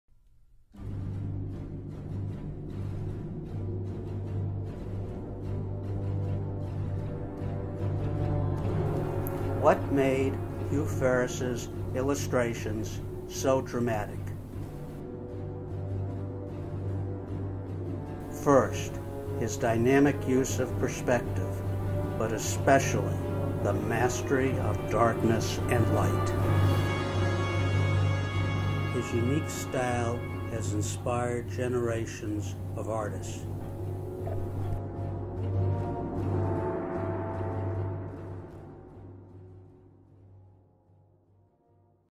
mfelderHughFerrissMiniDoc.mov
Mini-documentary in the style of Ken Burnes, made in After Effects by Merrick Felder at STLCC Meramec.